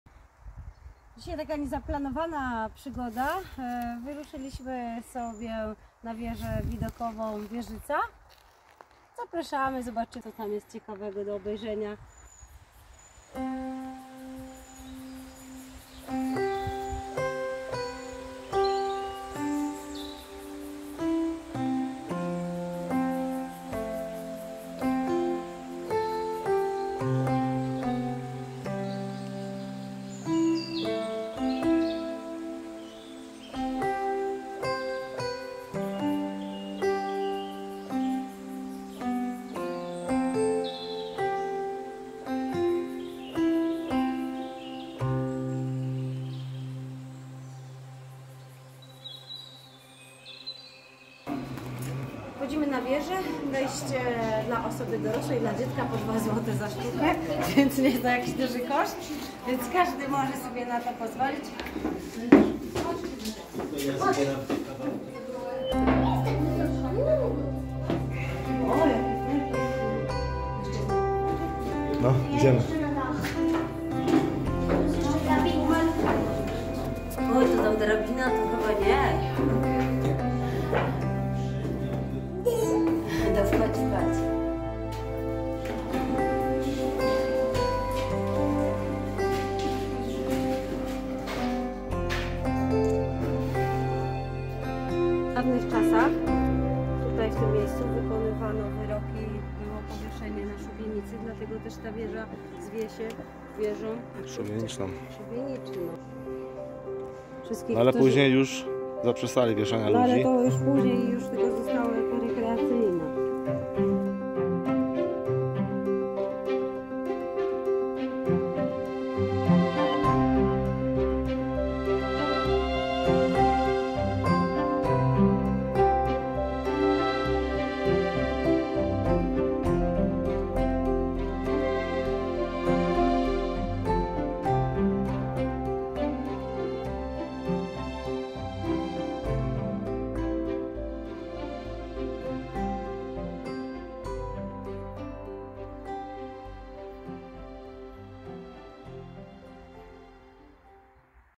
Wieża Widokowa na Wieżycy w Dobromierzu 395 m n.p.m.
Wieża widokowa na Wieżycy – wieża widokowa-pomnik na szczycie Wieżycy (395 m n.p.m.) na Pogórzu Bolkowskim.
 Na szczycie góry postanowiono wznieść pomnik, który miał być równocześnie wieżą widokową. Wieża została ukończona w 1852 roku, gdzie w późniejszym okresie przebudowano na Pomnik Chwały Żołnierza Pruskiego. Tuż przy wieży wybudowano restaurację, która działa do ok 1952 roku. Obecnie wieża jest w prywatnych rękach właściciela.